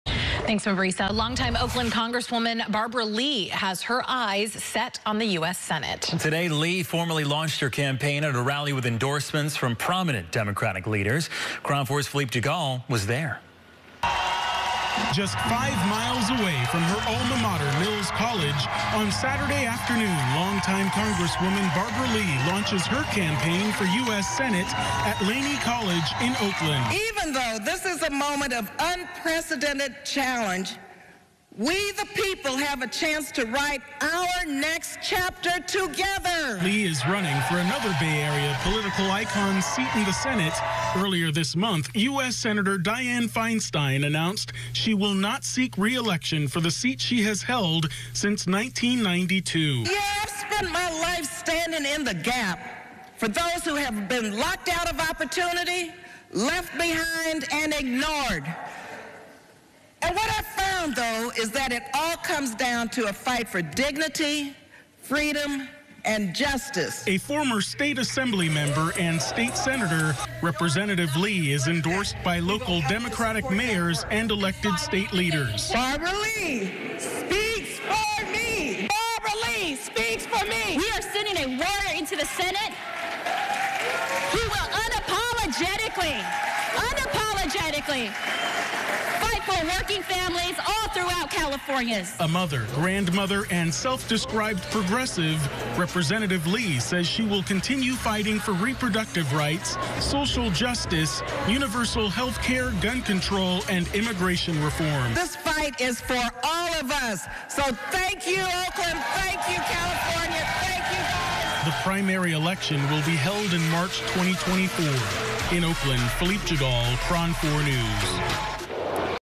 Rep. Barbara Lee launches campaign for US Senate
KRON4's Philippe Djegal reports.